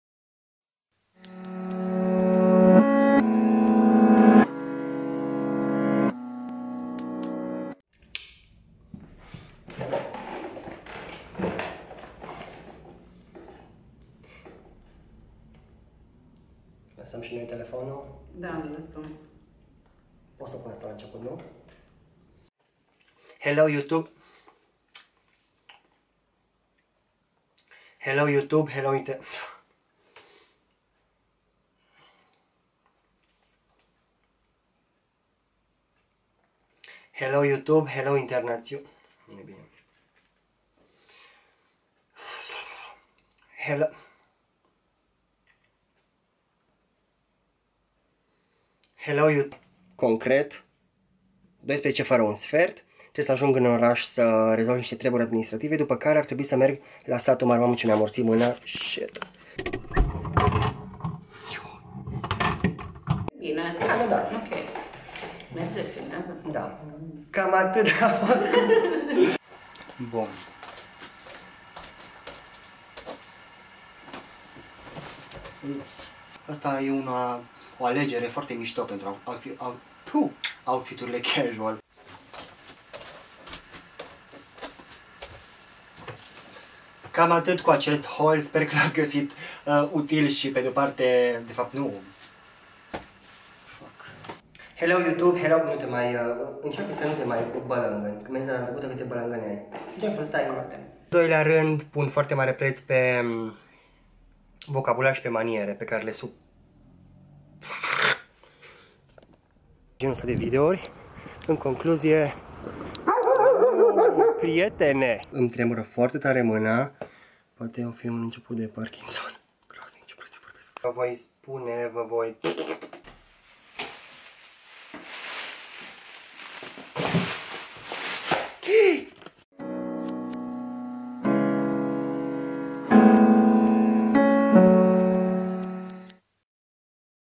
Bloopers | Momente amuzante la filmări
Creadeați că eu nu am momente din acestea? În video-ul de azi am selectat unele dintre cele mai amuzante faze de la filmari. Sper să vă amuzați și voi cel puțin la fel de mult pe cat m-am amuzat eu.

Mulțumesc în avans pentru vizionarea și totodată vă spun (scriu) că aștept părerile, sugestiile și toate cele în căsuța de comentarii.

Cu ce am filmat:
Nikon D3200